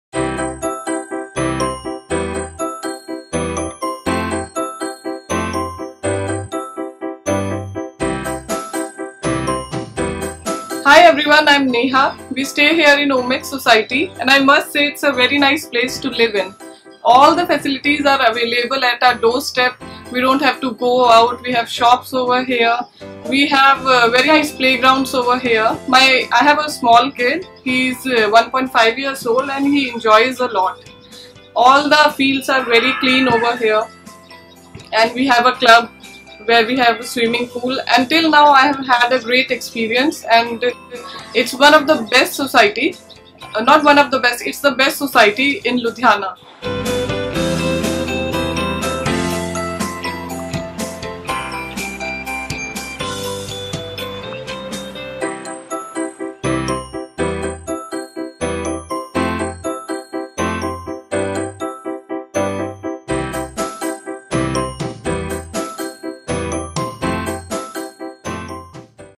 Royal Residency | Ludhiana - Omaxe
Here’s what our residents have to say about Royal Residency, Ludhiana. Get your hands on these top notch apartments and be a royal resident of Omaxe Ludhiana.